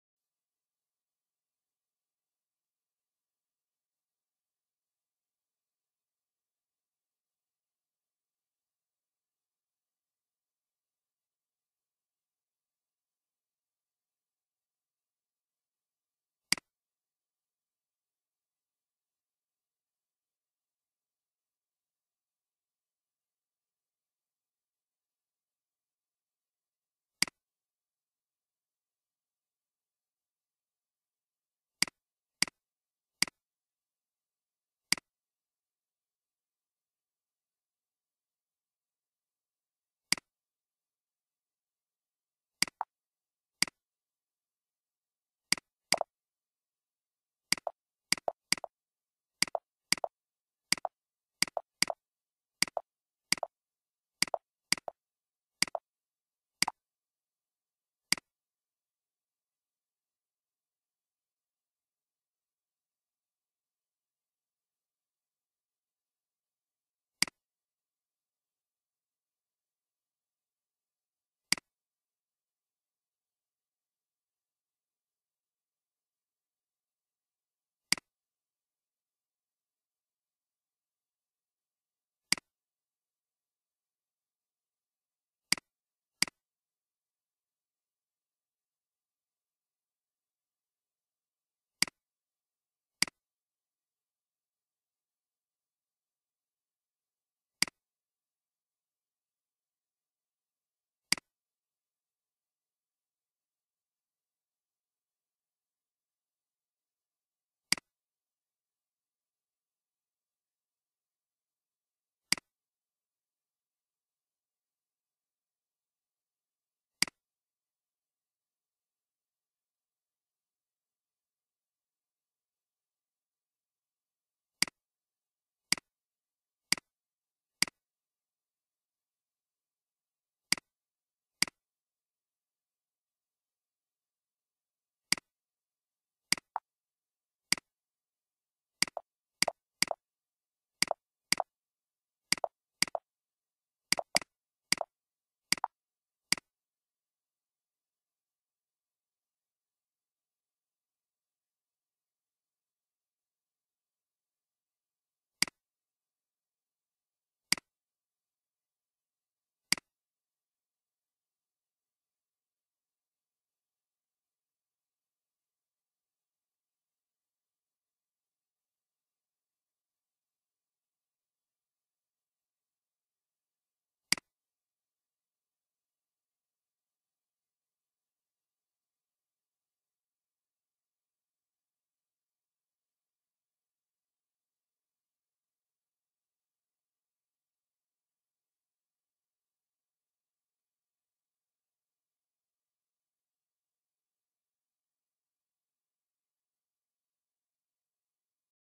sci hub أحسن وأسهل طريقة لتحميل الأبحاث العلمية والأوراق البحثية مجانا، النسخة العربية للموقع الشهير
: Sci hub رابط النسخة العربية للموقع الشهير